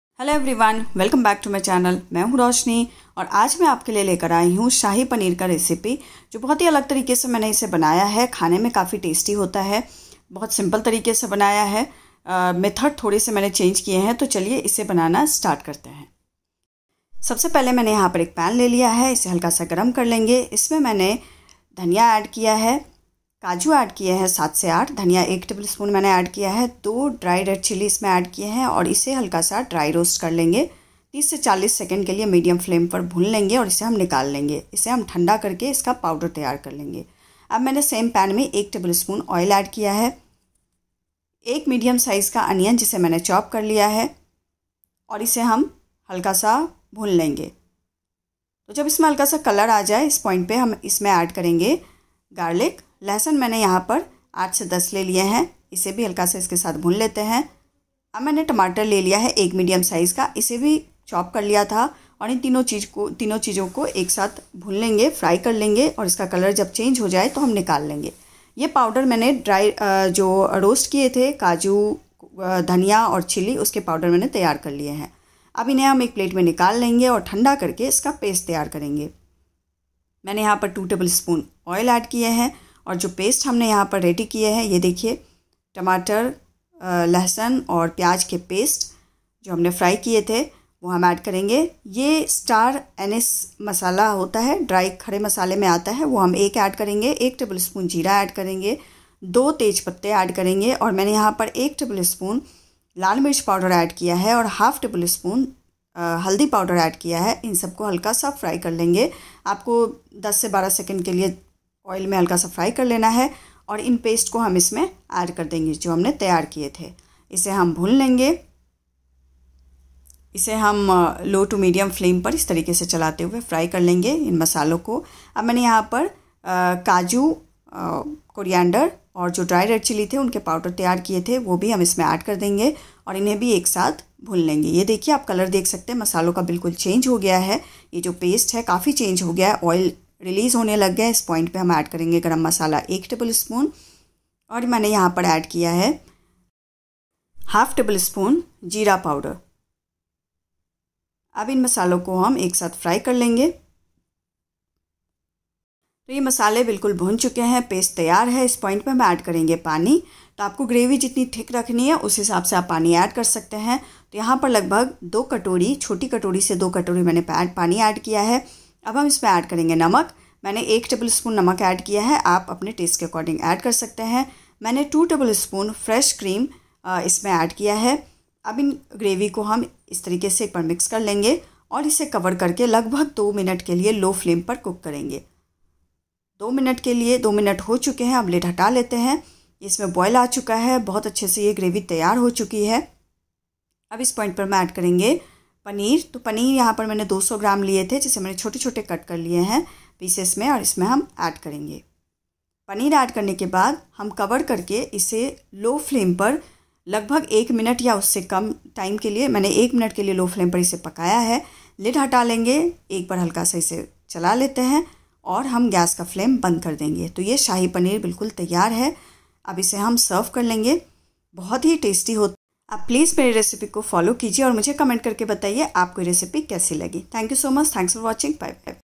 शाही पनीर बनाने का सबसे आसान और सही तरीका || shahi paneer recipe || easy and delicious
hello friends.
today i have presented shahi paneer recipe
i have cooked today in a very simple and easy way..it is cooked in a diffrerent method.
friends follow my method and prepare shahi paneer for your loved ones.

ingredients-
paneer-200g (cut in a small pieces )
whole coriander-1 tsp
cashew-7 to 8
red chilli-2